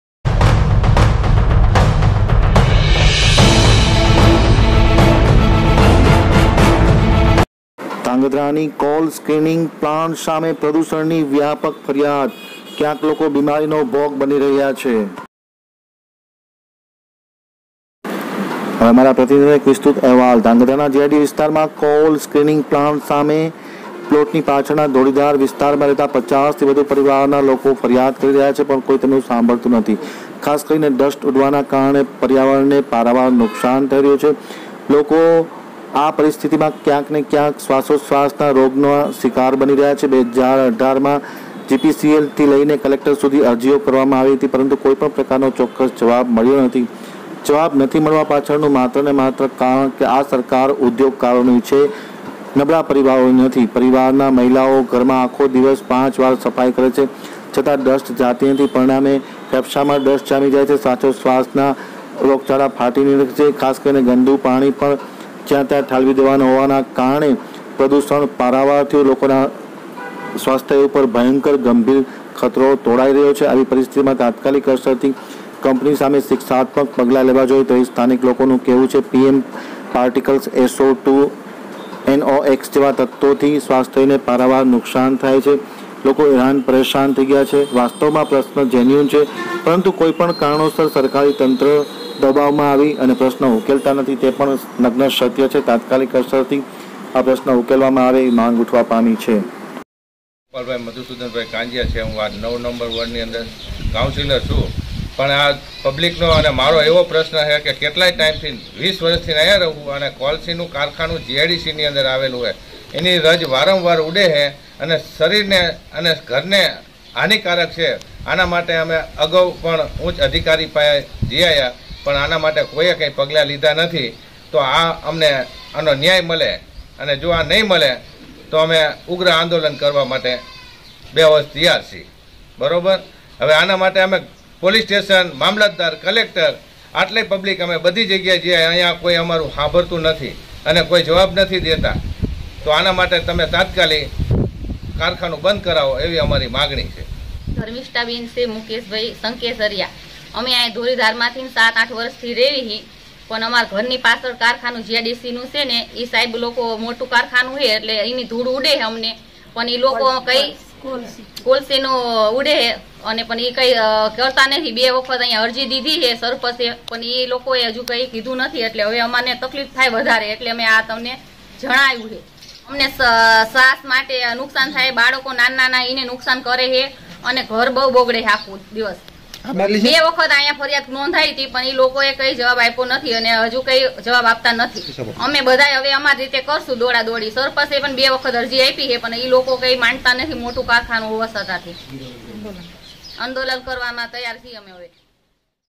AAMTAK_NEWS કોલ સ્ક્રીનિંગ પ્લાન્ટ ની ડસ્ટ બની સ્થાનિકોના માથાનો દુખાવો ભોગવે છે ભારે હાલાકી!
સ્લગ : કાળી કોલસી નો કકરાટ

સ્ટીકર :
(1) કોલ સ્ક્રીનિંગ પ્લાન્ટ ની ડસ્ટ બની  સ્થાનિકોના માથાનો દુખાવો.
(2) 50 ઉપર નાં સ્થાનિક પરિવારોએ સ્વાસ્થ્ય અને અન્ય પ્રશ્ન મુક્યા

ઉદ્યોગ દેશની શાન છે, વિકાસની હરણફાળ ભરવા ઉદ્યોગોને પ્રોત્સાહન આપવું જરૂરી છે. સરકાર અનેક સબસીડી, લોન, લોન માફી જેવા પગલાં થી ઉદ્યોગો ને પ્રોત્સાહન આપતી જ હોય છે. પણ દેશમાં પર્યાવરણ ને લગતાં 7 કાયદાઓ જે પર્યાવરણ અને પબ્લિકની સુરક્ષા માટે બન્યા છે એના પાલન સાથે ઉદ્યોગ ને વિકસાવવો એ પણ કાયદાકીય બાધિત જોગવાઈ છે.
પણ ઉદ્યોગ માત્ર વિકાસ નાં નામે જયારે બધા નીતિ નિયમ ભૂલે તયારે એના ઉપર યોગ્ય ન્યાય ની તલવાર પણ ફરવી જ જોઈએ.
      ધ્રાંગધ્રા નાં GIDC વિસ્તારમાં કોલ સ્ક્રીનિંગ પ્લાન્ટ સામે પ્લોટ ની પાછળનાં ધોળીધાર વિસ્તારમાં રહેતા 50 થી વધુ પરિવારના લોકો ફરિયાદ કરી રહ્યા છે, પણ કોઈ એમનું સાંભળતું નથી એવા એમના આક્ષેપ છે. 2018 માઁ gpcb થી લઇ ને કલેક્ટર સુધી અરજીઓ કરેલી છે જેના કોઈ વળતા જવાબ તો નથી પણ સામે કોઈ ચોક્કસ સુધાર પણ નથી.
પરિવાર નાં મહિલાઓ ઘર માઁ આખા દિવસમાઁ 5 વાર પોતા અને ઘર ની સફાઈ કરી કરી ને ત્રસ્ત બની છે. બાળકોના સ્વાસ્થ્ય માટે આ પરિવારો ચિંતિત છે. ઘર ની દીવાલો, વાસણ, પંખા તમામ જગ્યા એ કોલસી ની કાળી ડસ્ટ થી એમને પારાવાર મુશ્કેલીઓ પડી રહી છે.
મહત્વની વાત એ છે કે ધ્રાંગધ્રા શહેરમાં આ પ્લાન્ટ 3 જગ્યાએ લોકોની ફરિયાદ બાદ જગ્યા બદલી બદલી ને અહીં નાખવામાં આવ્યો છે, તો જવાબદાર તમામ ખાતા ને આ વાત ધ્યાને કેમ નથી આવી એ જ મૂંઝવણ સમાન પ્રશ્ન છે. પ્લાન્ટ માઁ વોટર ડિસ્ચાર્જ ઝીરો બતાડે છે પણ પાછળ ખુલ્લે આમ ગેરકાયદેસર ગંદુ પાણી ફ્લો થાતું દેખાય છે. પ્લાન્ટમાં ડસ્ટ કંટ્રોલ માટે સારી કક્ષાનું ઇનપુટ હોપર ફરજીયાત હોવું જોઈએ પણ એના બદલે પતરાં થી ઢાંકવાનું કામ આ કોલસી અમારા સુધી પહોંચતા અટકાવી નહિ શકાય એમ સ્થાનિકોનું સ્પષ્ટ ક્હેવું છે. Pm પાર્ટિકલ્સ, SO2, NOX જેવા તત્વો સ્વાસ્થ્ય ને ખાસ કરી ને ફેફસા ને નબળા પાડી શકે છે.
બાઈટ :
(1) ગોપાલ કાંજીયા (ચાલું bjp સુધરાઈ સભ્ય વોર્ડ 9)
(2) સ્થાનિક મહિલા

લોકોની ફરિયાદ જોતા કંપની નાં મેનેજર ને પ્રશ્ન કરતા એમનું કહેવું એવું છે કે સરકારે GIDC માઁ પ્લોટ ફાળવેલ છે ડસ્ટ તો આમાં ઊડતી હોય પણ અમે બને એટલા પ્રયત્ન કરીશું.
હાલમાં જ સેવાકીય કાર્યમાં મોખરે રહેતા GIDC પ્રમુખ રાજુભાઈ એ ધ્રાંગધ્રા GIDC માઁ નાના ઉદ્યોગો છે અને કોરોનમાં હાલ બેહાલ છે, તમામ ઉદ્યોગ મૃતપાય છે જેથી સરકારે યોગ્ય સહાય કરવી જોઈએ એવું નિવેદન આપ્યું છે જેને સરકારે ચોક્કસથી ધ્યાનમાં લેવું જોઈએ. ધ્રાંગધ્રા GIDC માઁ માંડ 8 નાના ઉદ્યોગ બચ્યા છે જેના ઉપર આજ સુધી કોઈ ફરિયાદ પણ નથી તો એમને મજબૂત બનાવવા યોગ્ય પગલાંઓ પણ લેવાવા જોઈએ. પણ આવા પ્લાન્ટ જ્યાં સ્થાનિકો ની ફરિયાદ હોય ત્યાં પણ લાલઆંખ કરવી જોઈએ
વિજય ઘેલાણી આમ તક ન્યૂઝ ભુજ ૯૭૧૨૯ ૯૭૩૦૯